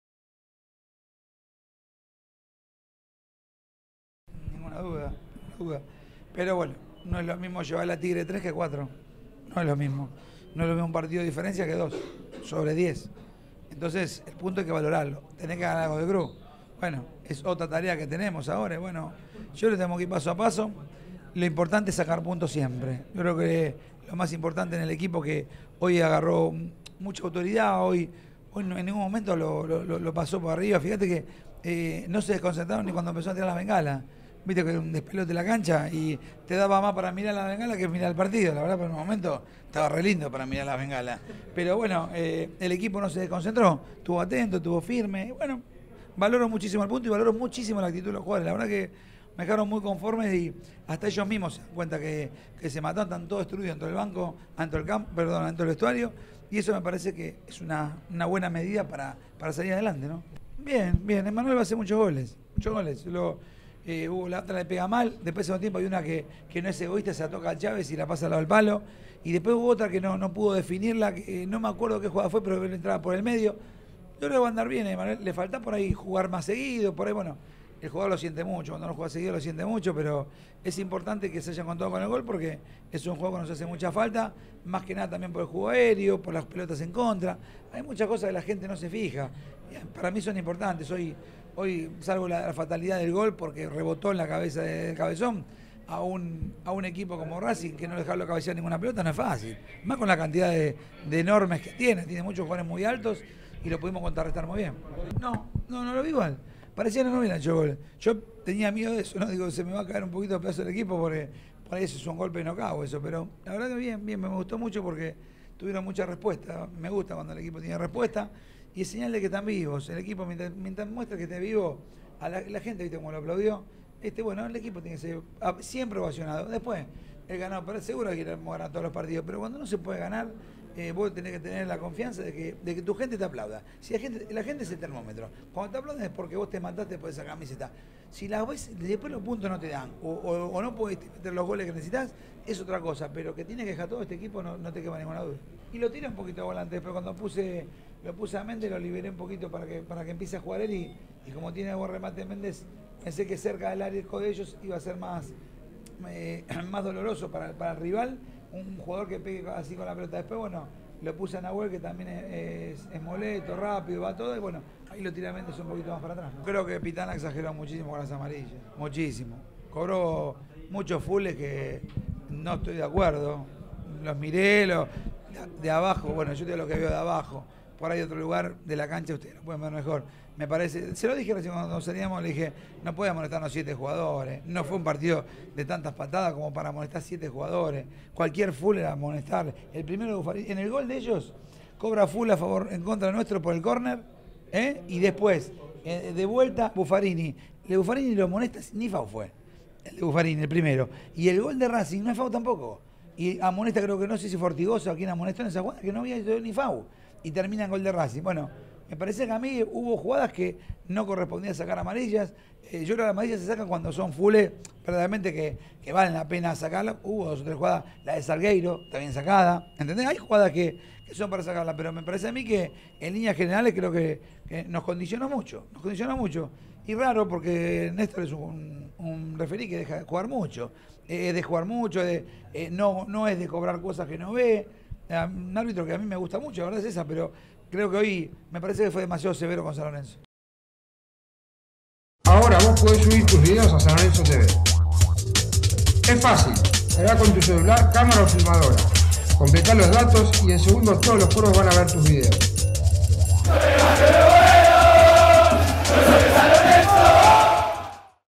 Racing 1 San Lorenzo 1 - Conferencia Caruso Lombardi parte 2 - San Lorenzo TV
En una sala de prensa ínfima y con medios de todo el país esperándolo, se hizo presente en este sector el flamante técnico de San Lorenzo de Almagro, Ricardo Caruso Lombardi, luego del empate ante Racing en Avellaneda. Rescató que "no se desconcentraron ni cuando empezaron a tirar las bengalas", "te daba más para mirar las bengalas que para mirar el partido" disparó un Caruso ocurrente quien arranca las sonrisas cada vez que declara. Mirá el video con sus palabras.